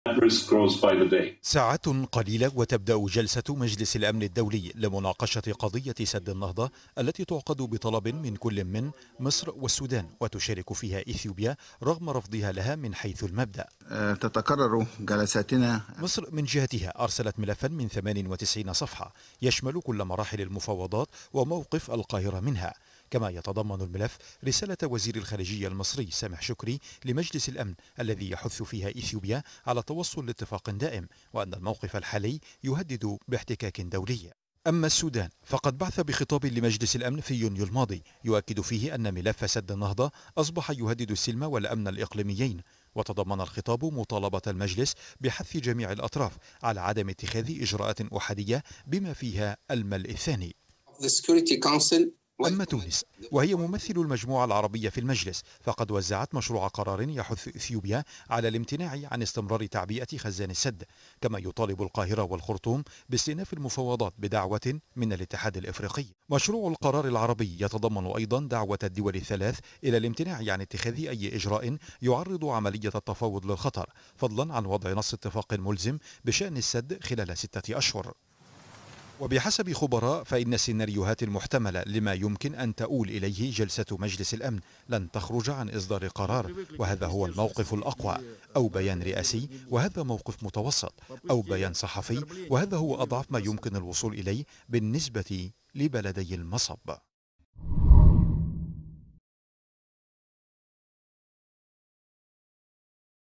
بين إصدار قرار ملزم أو بيان صحفي.. ملامح من جلسة مجلس الأمن بشأن سد النهضة
أكد وزير الخارجية المصري سامح شكري، أن بلاده ستبحث في مجلس الأمن المطالبة بالتوصل لاتفاق ملزم بشأن سد النهضة في غضون ستة أشهر، مشيرا إلى أن عشرة أعوام من المفاوضات فشلت بضمان استمرار تدفق مياه النيل الأزرق لمصر والسودان،

من جانبه دعا رئيس الوزراء الإثيوبي أبي أحمد لدعم جهود التعبئة الثانية لسد النهضة، معتبرا ذلك دفاعا عن  السيادة والدبلوماسية الإثيوبية على حد قوله.